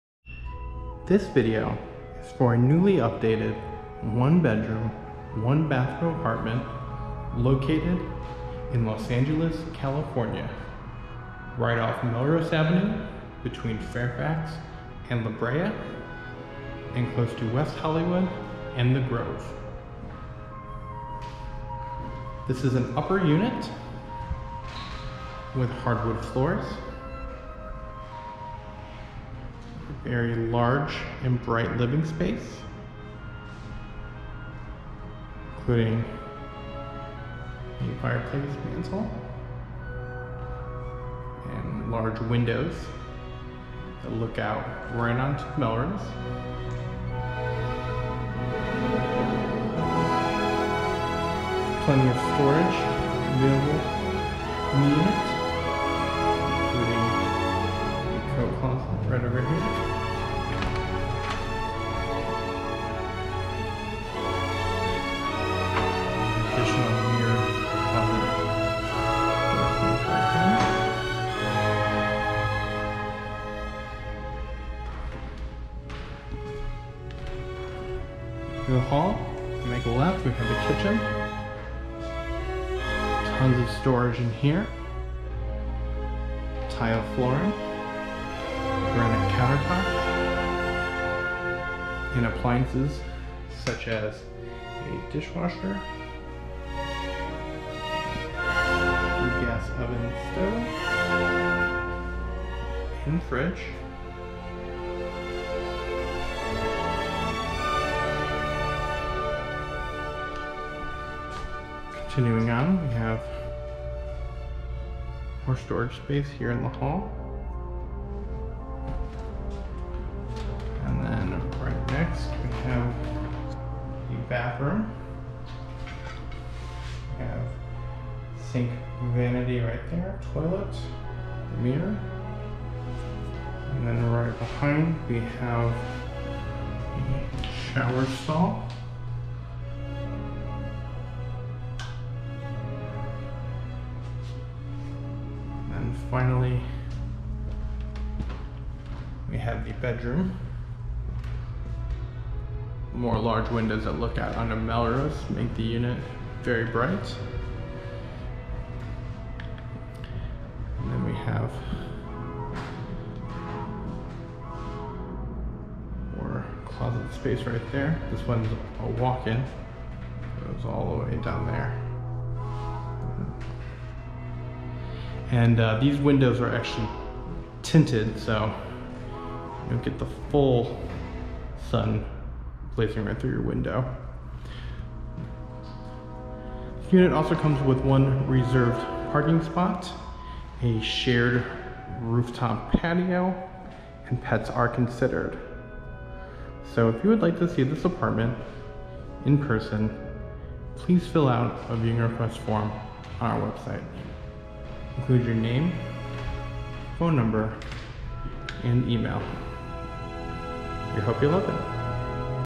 PL9581 - Unique Upper 1 Bed + 1 Bath Apartment For Rent (West Hollywood, CA)
Pacific Listings is proud to present this TRULY UNIQUE 1+1 Unit Above Melrose Shopping! Updated w/ Hardwood Floor, A/C, Kitchen Appliances, Rooftop Patio, Laundry On-Site & Parking!